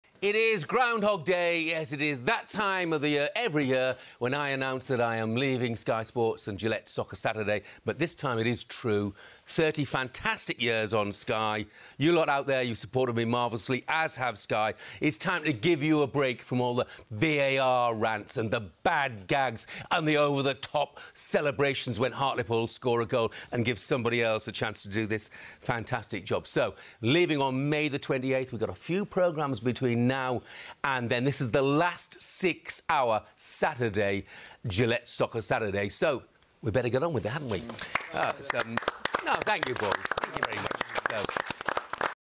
Jeff Stelling to leave Soccer Saturday after more than 25 years t helm of Sky Sports show
Jeff Stelling has announced he will be leaving Soccer Saturday at the end of the season after over 25 years at the helm of the show.

The 68-year-old revealed his intention to step down live on air during this weekend's Soccer Saturday - a show he has helped make a fixture for football fans in the UK for more than 25 years as its award-winning presenter.